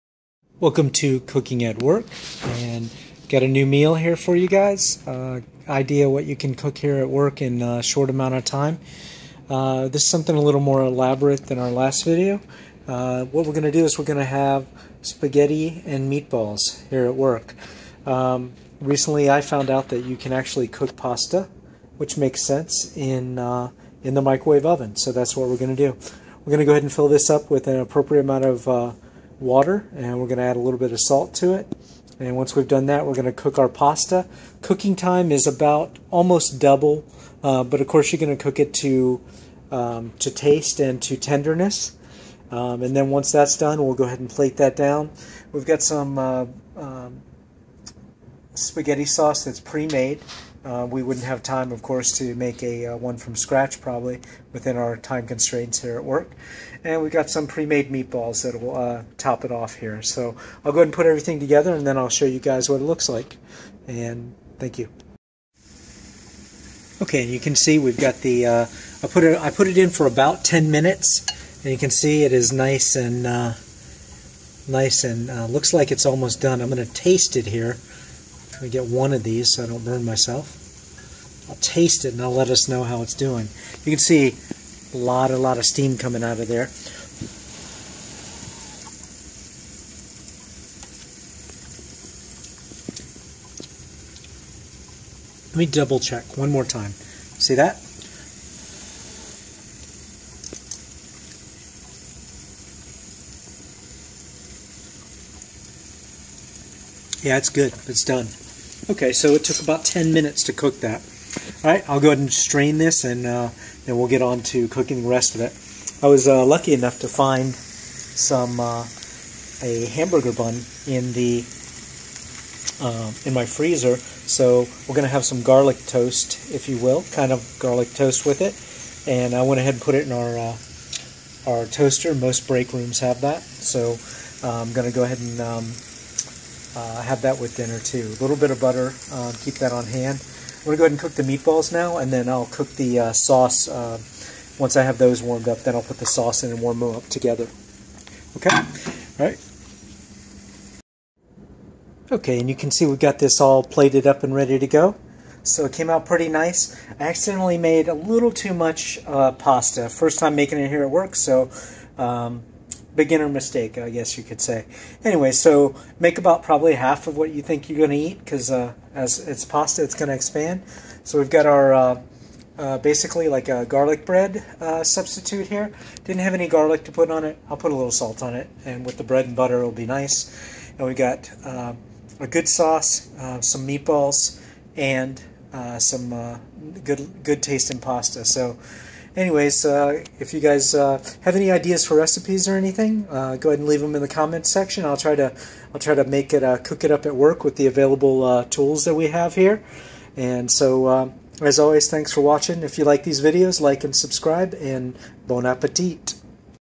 Cooking At Work Episode 2
Episode 2 in our cooking at work series.
Today's episode is Italian themed and included angle hair pasta and red sauce and meatballs. I left over hambuger bun is utilized for a garlic bread substitute. I fould some left over garlic salt in the office and was able to top it of right.
Overall this meal was delicious and only took about 15 min to make fresh!